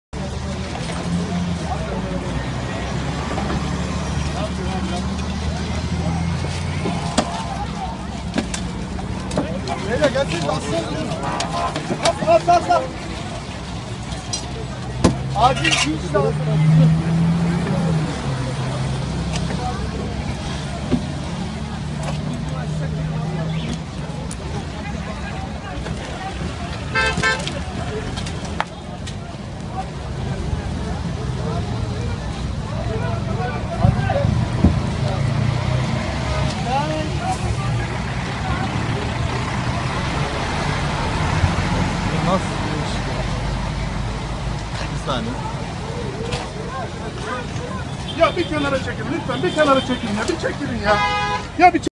İstanbul Acıbadem Durağında Yaşanan Metrobus Kazasından İlk Görüntüler - Haber10 TV
Haber10 TV Resmi YouTube Kanalı